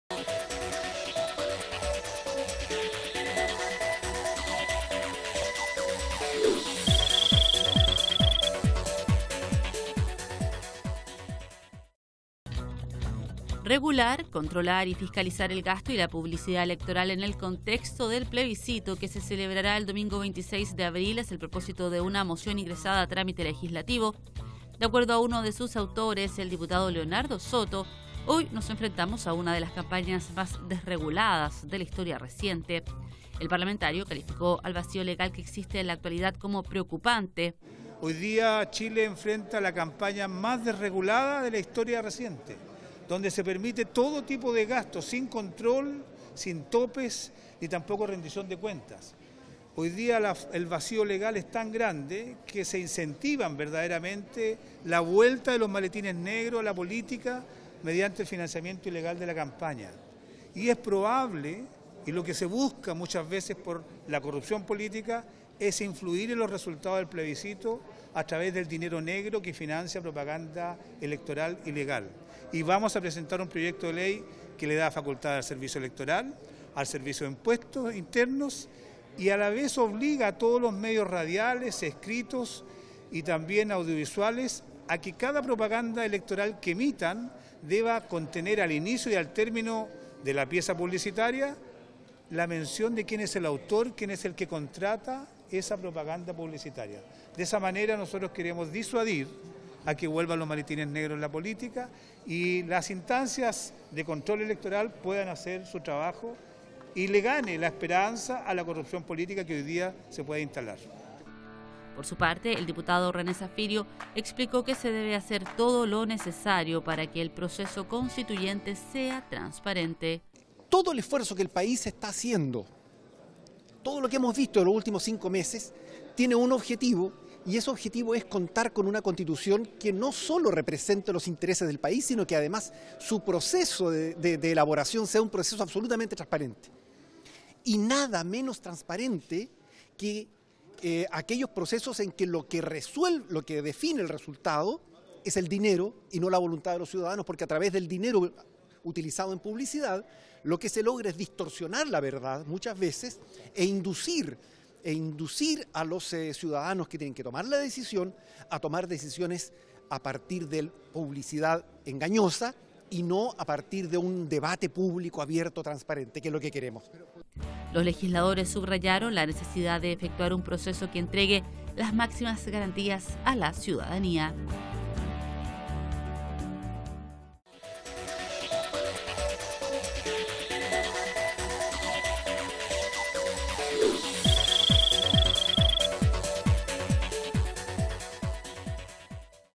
Presentan proyecto para regular y fiscalizar gasto electoral
Un grupo de parlamentarios de la comisión de Constitución se refirió a la iniciativa, señalando que hay vacíos legales que permitirían el financiamiento irregular de las campañas del plebiscito de abril próximo.